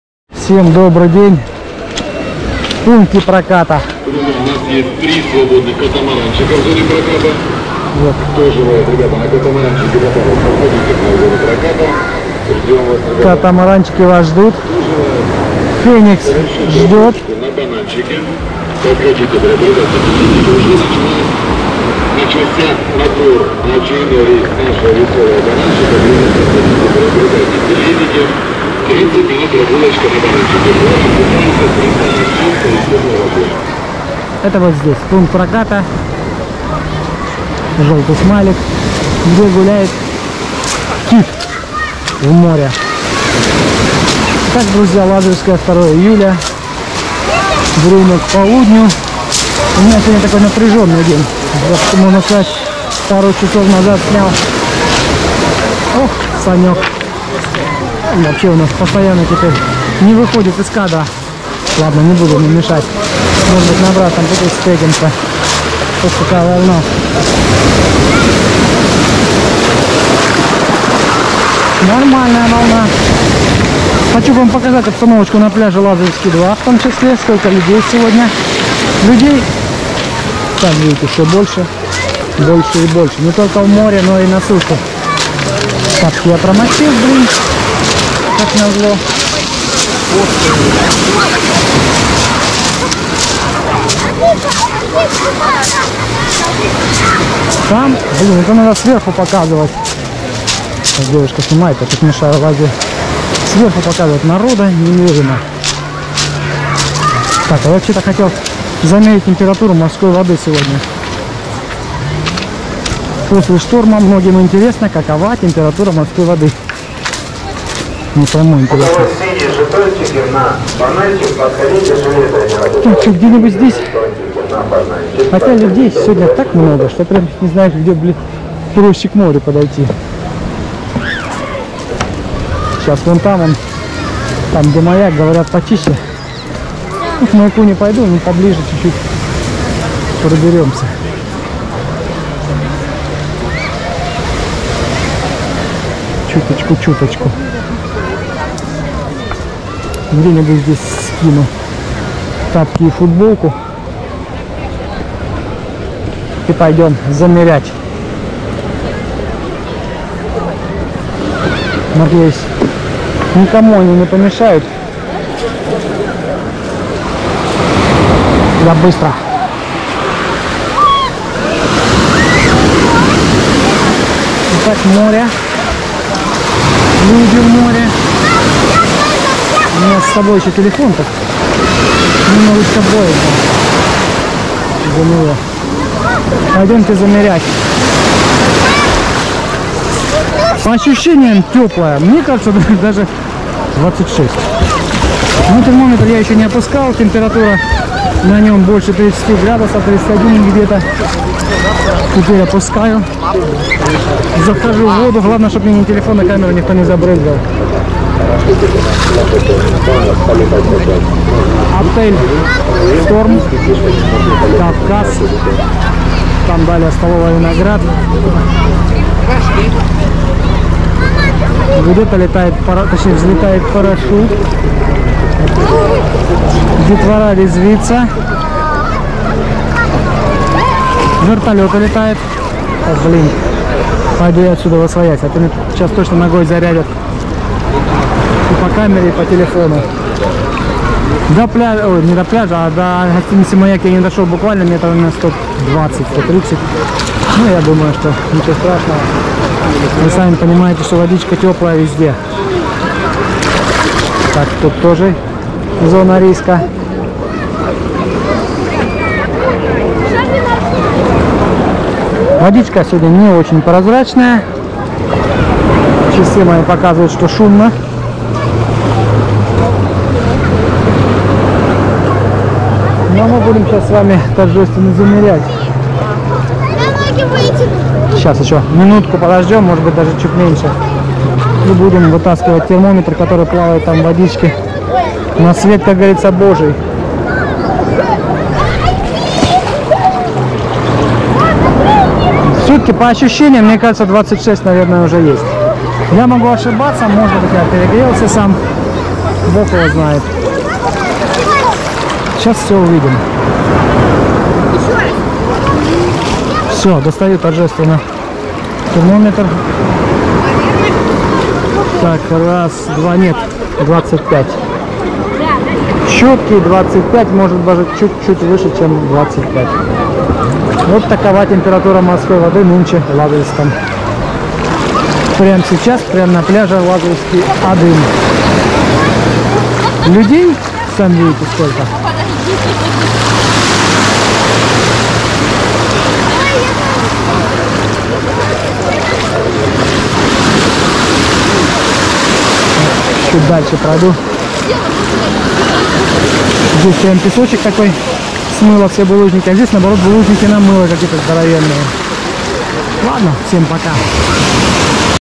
Замеряю море после шторма! Лазаревское 02.07.2023. Людей уже много!🌴ЛАЗАРЕВСКОЕ СЕГОДНЯ🌴СОЧИ.
Вся информация по жилью "АРКА","Владимир" Кавртира на ул.Победы. 83.